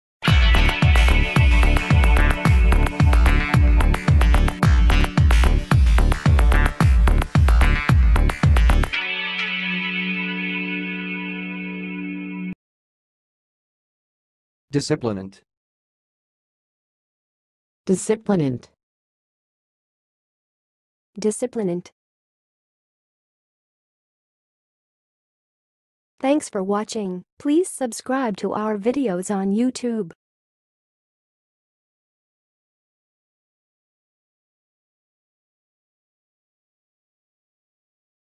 How to say "disciplinant"! (High Quality Voices)
Watch in this video how to say and pronounce "disciplinant"!